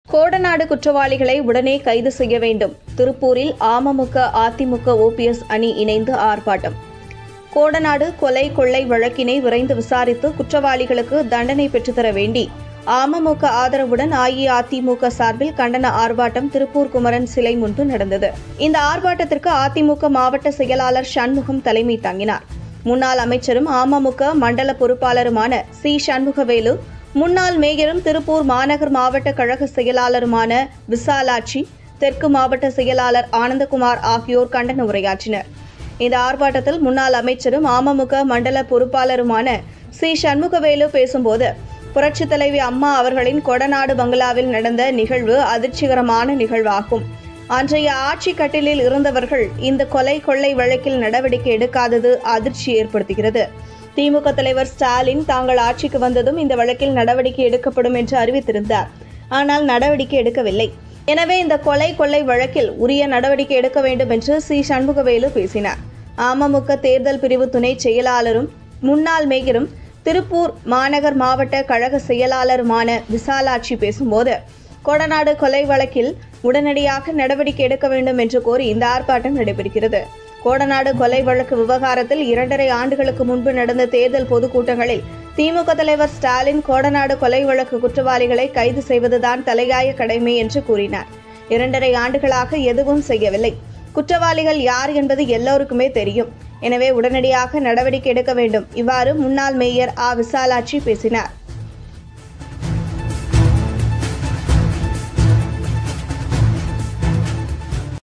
திருப்பூரில் அமமுக, அதிமுக ஓ.பி.எஸ். அணி இணைந்து ஆர்ப்பாட்டம்
கோடநாடு குற்றவாளிகளை உடனே கைது செய்ய வேண்டும் என திருப்பூரில்
அமமுக, அதிமுக ஓ.பி.எஸ்., அணி இணைந்து ஆர்ப்பாட்டம்